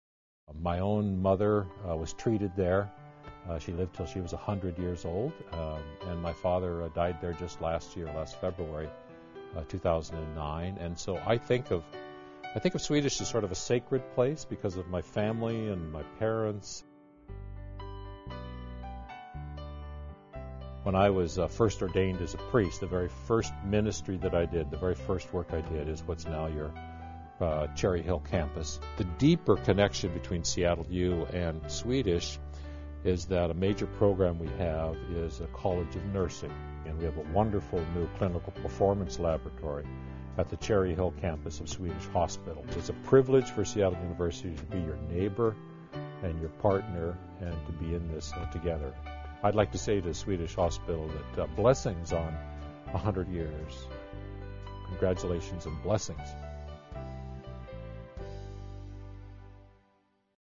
Swedish 100 - Fr. Stephen Sundborg, S.J., President, Seattle University
Swedish and Seattle University not only share a neighborhood, but also a long tradition of working together to advance the cause of nursing. Learn about the Seattle University Clinical Performance Lab